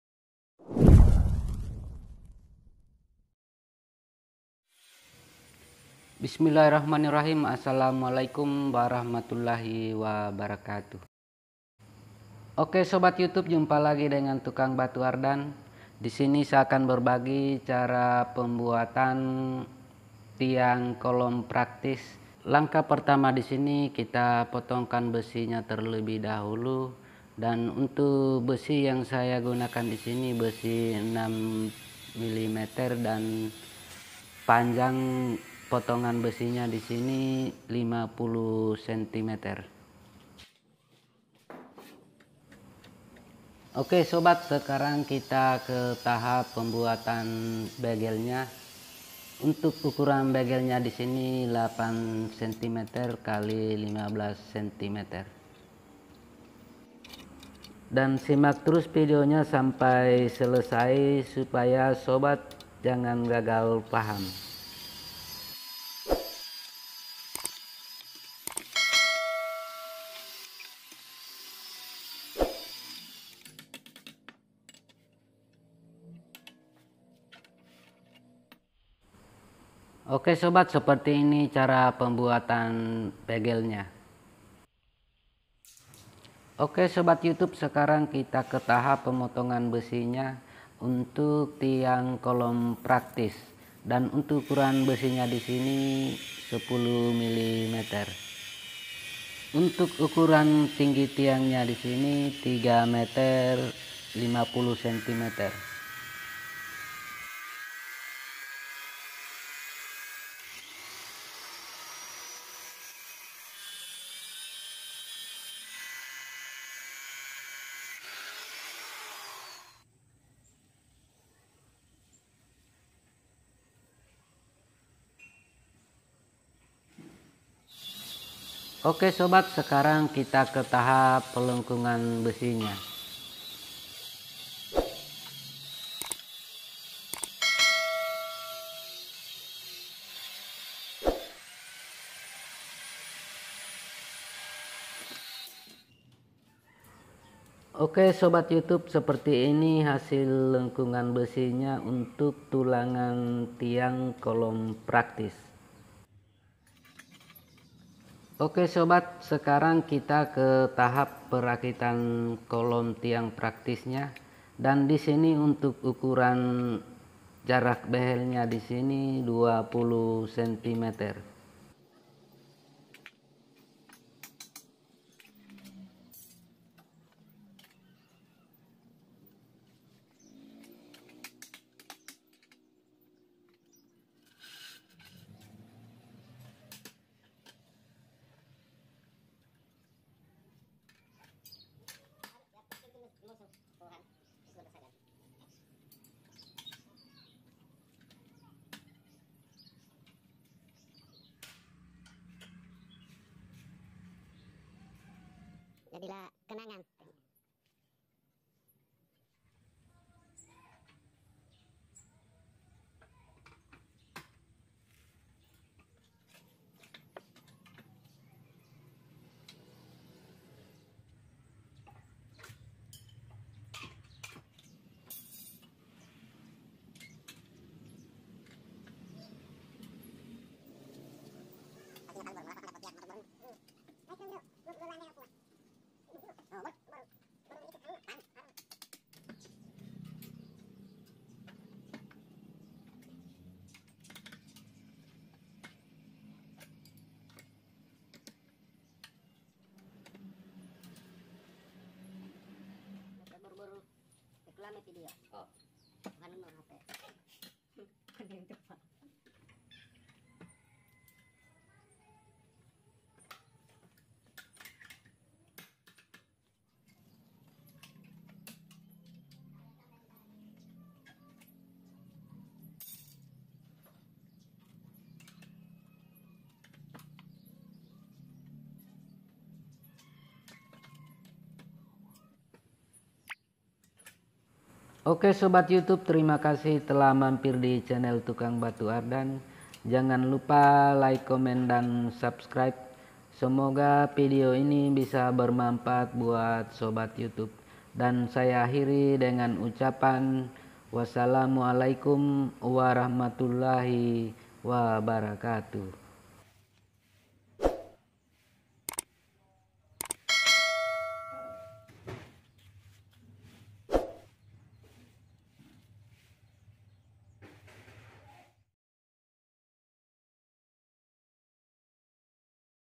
Cara merakit besi tiang cor rumah cepat dan rapi@tukangbatuardan
assalamuallaikum, dividio saya ini membahas tentang cara merakit tiang kolom beton rumah, semoga vidio ini bisa bermamfaat buat sobat youtube.
bahan yang digunakan besi 10 mm dan besi 6mm, terimakasih. yuk subscribe chanel youtube tukang batu ardan dan aktifkan lonceng supaya sobat dapat notifikasi vidio terbaru dari tukang batu ardan terimakasih.